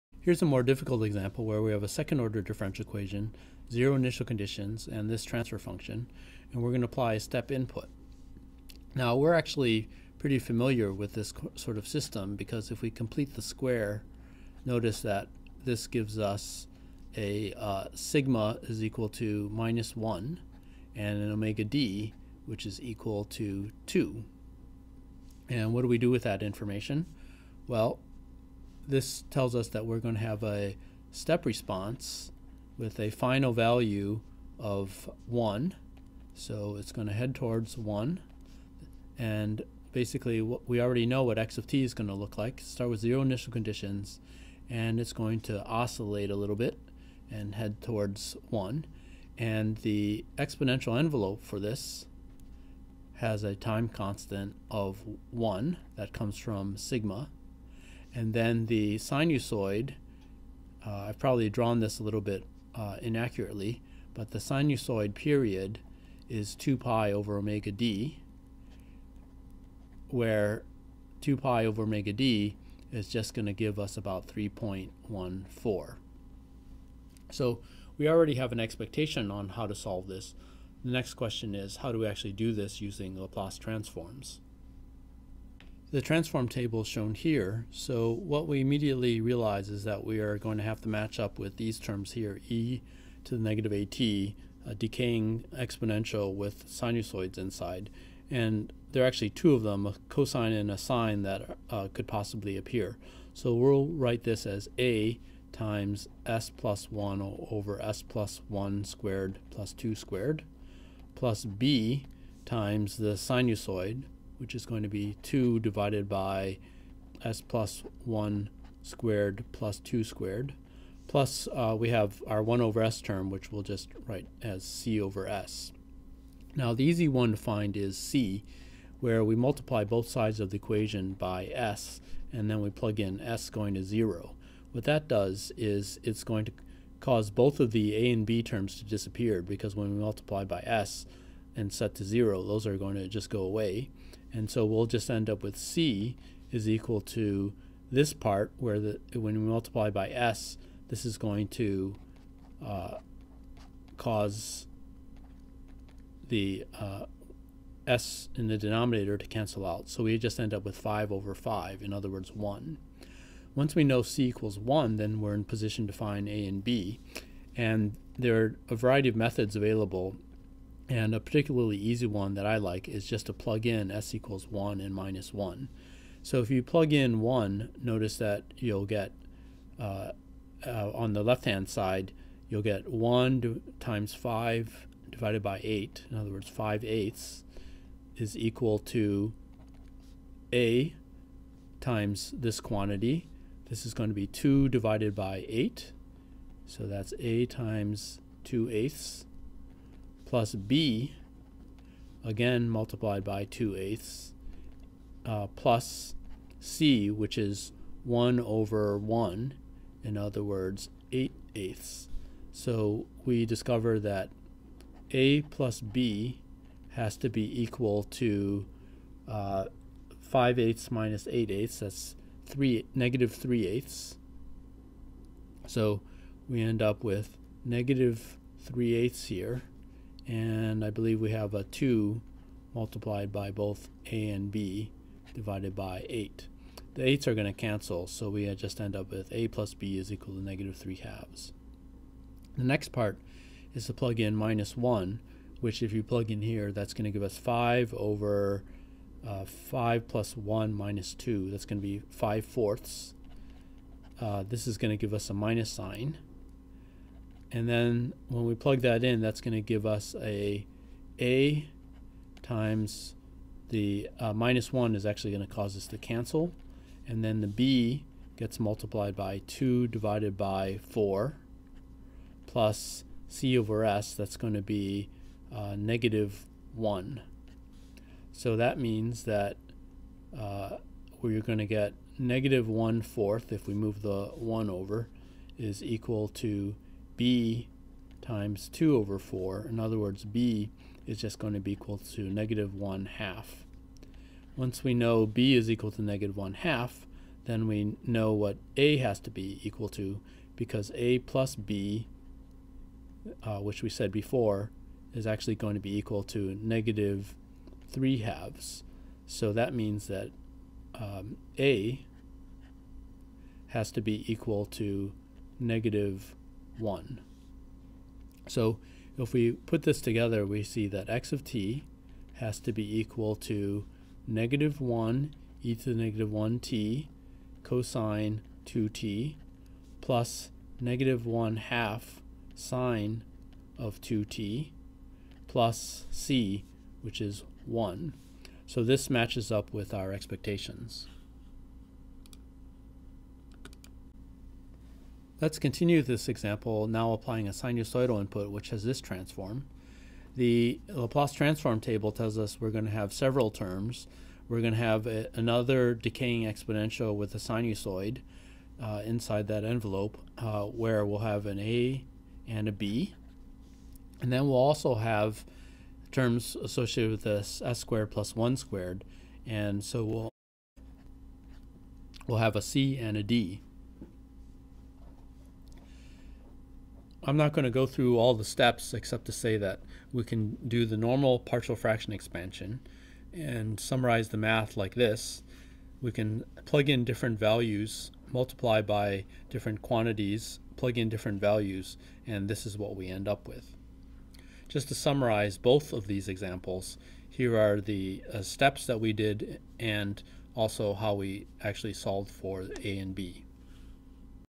Laplace example solve second order
Table of Contents:

01:25 - Example 2A
05:44 - Example 2B Video supplementary lectures from "Modeling, Analysis, and Control of Dynamic Systems," ME 360 Winter 2015. Supplementary video lectures for "Modeling, Analysis, and Control of Dynamic Systems," ME 360 Winter 2015, at University of Michigan.